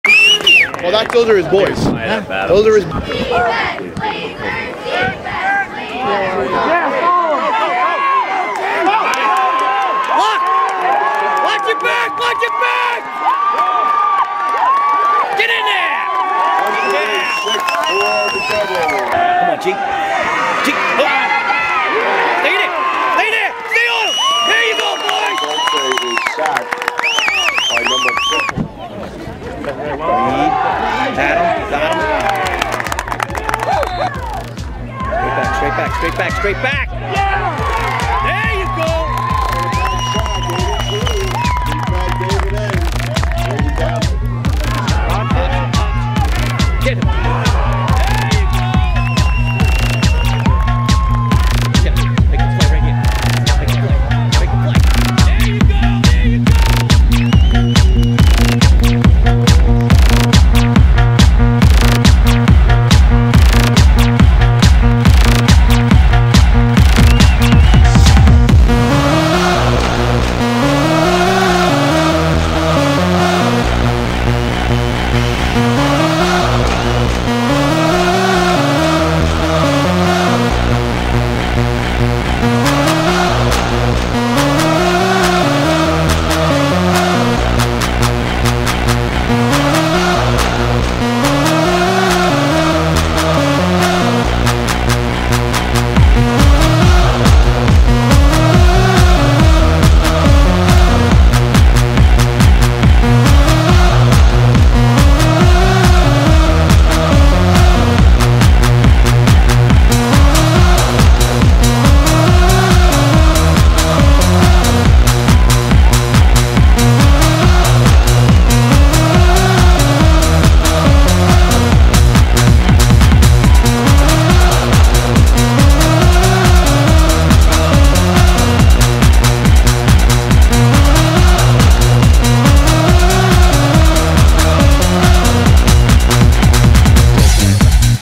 PLAINEDGE FOOTBALL 2009 1ST ROUND
Plainedge Varsity Football defeats Great Neck North 21 -13 - Off to Hofstra!